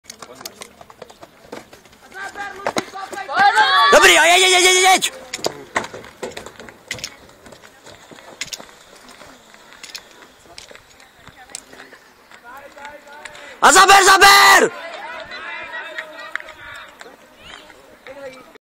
Okresní kolo dorostu Bludov 27.04.2013 Maistryszin 100m překážek
Záznam běhu 100m překážek a dvojboj jednotlivci SDH Třeština.